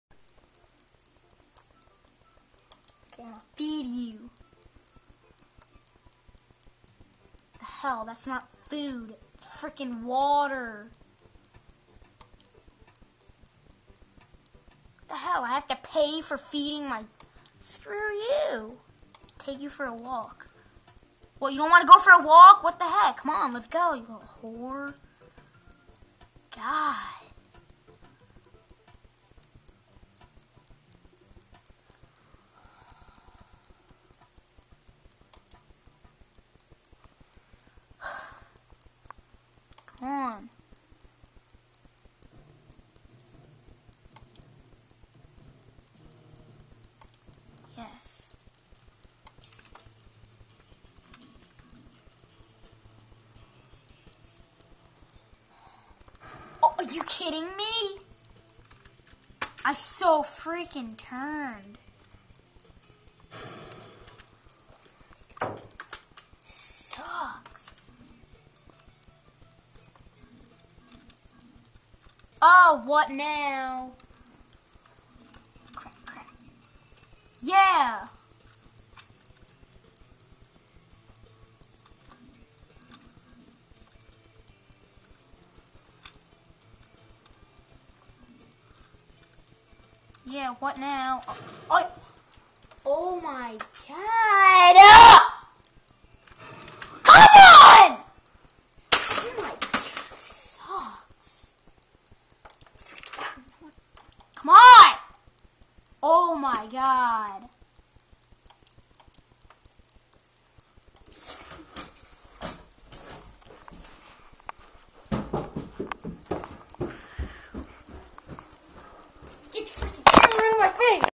Great Freakout 1: Puffle feeding
stephen freaksout about feeding puffles NOT FAKE